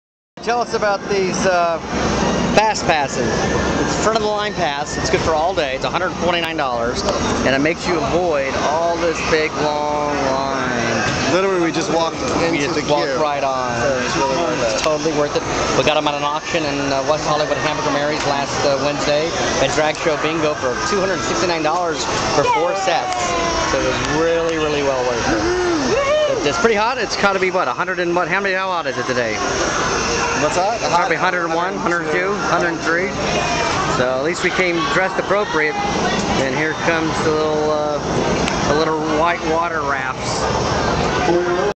SHAW STORIES - Universal Studio's Tour Jurassic Park Ride "Front of Line Pass" - AUG. 13, 2009
Universal Studio's Tour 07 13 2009 Jurassic Park Ride "Front of Line Pass"